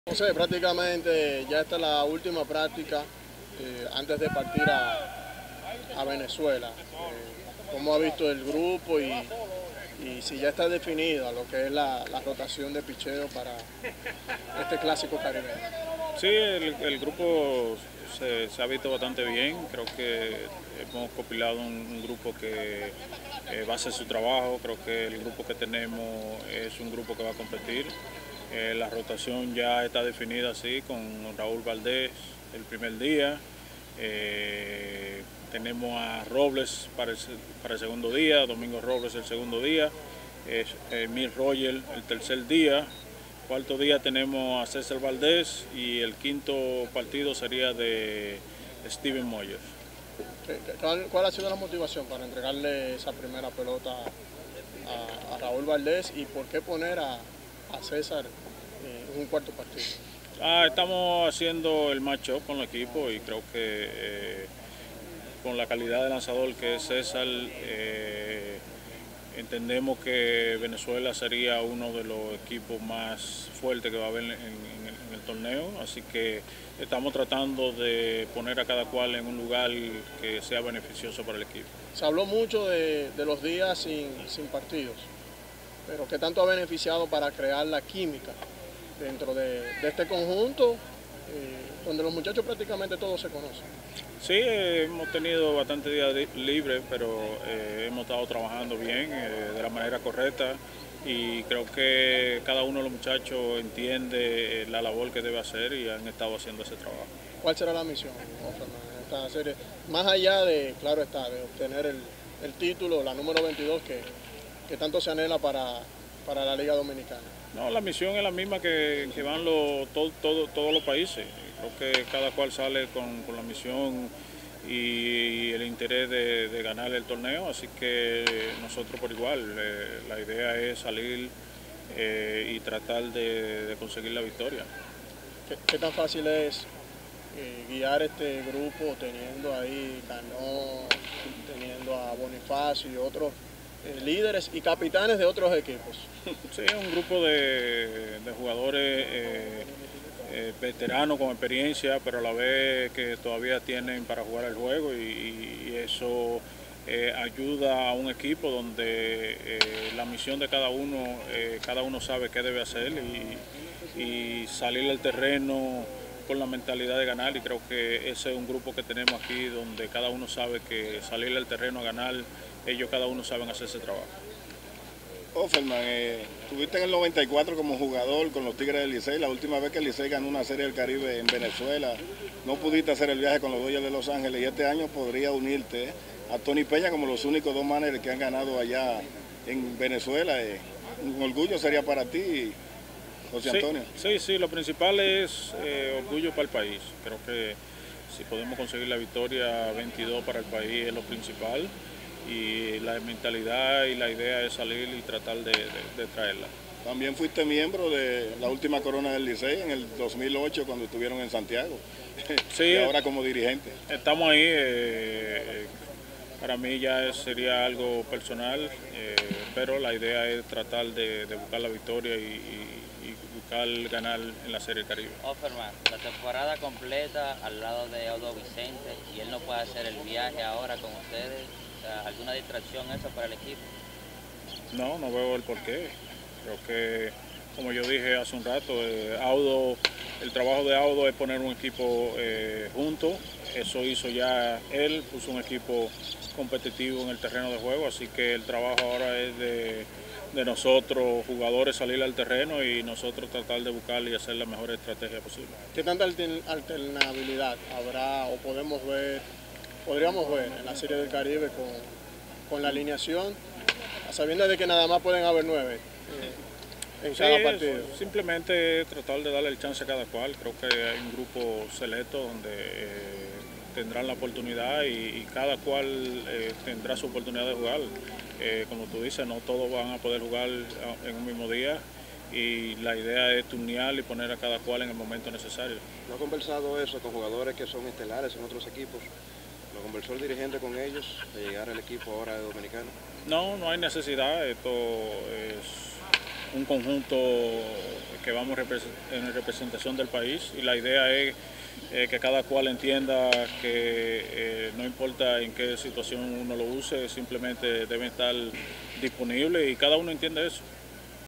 JOSÉ OFFERMAN: "CONTAMOS CON EL PITCHEO NECESARIO PARA GANAR"
El dirigente de los Tigres del Licey, José Offerman, y su staff de coaches definieron la rotación de lanzadores de cara a la participación del equipo en la Serie del Caribe, que iniciará el jueves.

“Todos nuestros pitchers han trabajado todos estos días para mantenerse en forma y sus brazos han lucido bien, tanto en los bullpens que han lanzado y en los partidos inter escuadra que hemos tenido”, destacó Offerman, quien aseguró: “Contamos con el pitcheo necesario para ganar”.

El capataz de la escuadra representante de la República Dominicana manifestó que sus cinco serpentineros iniciales serán Raúl Valdés, Domingo Robles, Esmil Rogers, César Valdez y Steven Moyers.

Valdez tendrá el compromiso contra Venezuela y Moyers se medirá a Panamá. “Hemos evaluado a los diferentes oponentes y nos hemos motivado a realizar la rotación de esta forma”, expresó Offerman, quien no dejó claro si utilizará a los dos primeros abridores para enfrentar a Colombia y Curazao, señalando que el equipo cuenta con varios lanzadores que pudieran iniciar esos encuentros.

Sobre Valdez, explicó las razones del porqué el Lanzador del Año de la presente temporada será el abridor para el cuarto compromiso.

Offerman resaltó las condiciones en las que se han mostrado todos los lanzadores durante la semana de entrenamientos que ha tenido el equipo previo a su partida a Venezuela mañana miércoles.

Respaldó las declaraciones del coach de pitcheo Erick Abreu, al señalar que el plan de trabajo de los lanzadores no será muy diferente al que tuvieron durante toda la temporada.